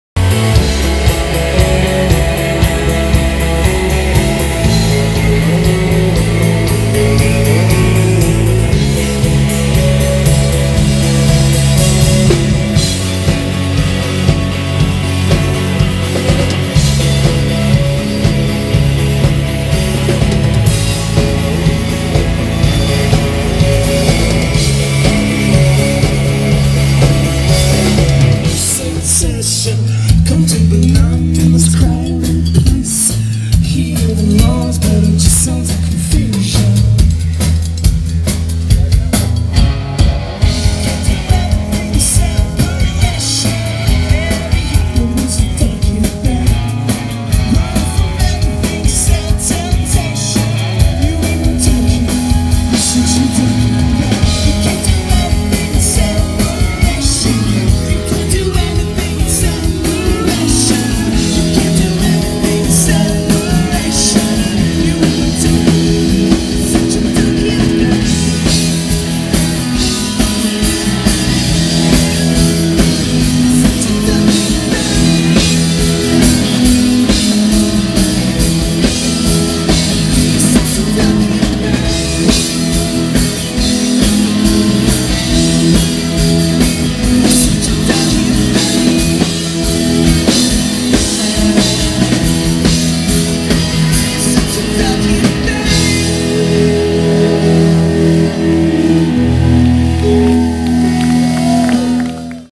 Mossy Scion Concert Series March
This was the first of 4 concerts in the 2011 Mossy Scion Concert Series.  Took place on March 24th and featured two terrific local bands, the Lyon Crowns and Dynamite Walls!